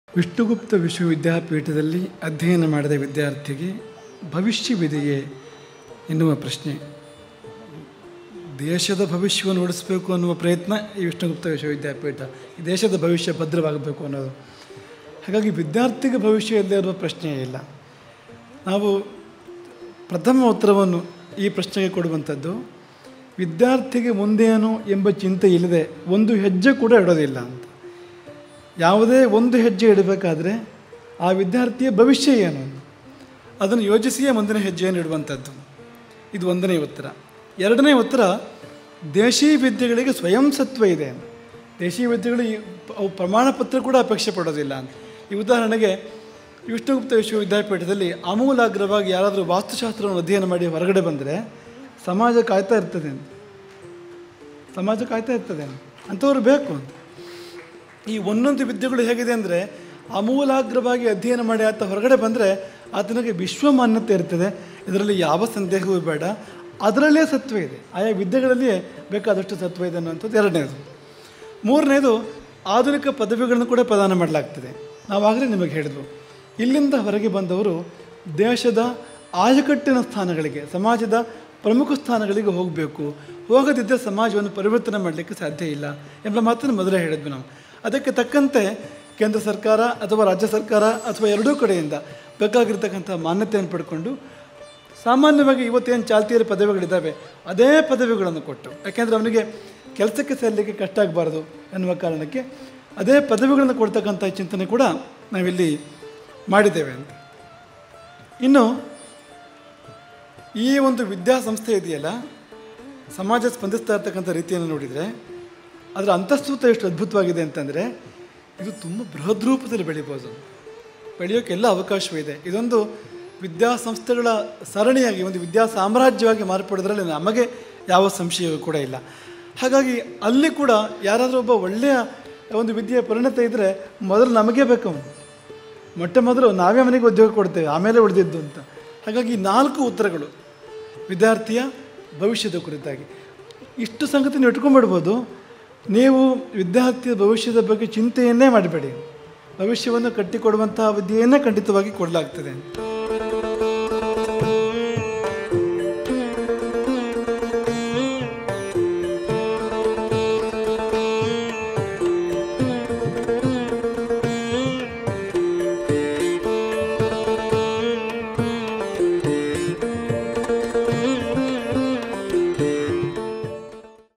ವಿಷ್ಣುಗುಪ್ತ ವಿಶ್ವವಿದ್ಯಾಪೀಠದಲ್ಲಿ ಅಧ್ಯಯನ ಮಾಡಿದ ವಿದ್ಯಾರ್ಥಿಗೆ ಭವಿಷ್ಯವಿದೆಯೇ?
Location: Ashoke, Gokarna
Vishnugupta Vishwa VidyaPeetha

Srimad Jagadguru Shankaracharya Sri Sri Raghaveshwara Bharati Mahaswamiji Sri RamachandrapuraMatha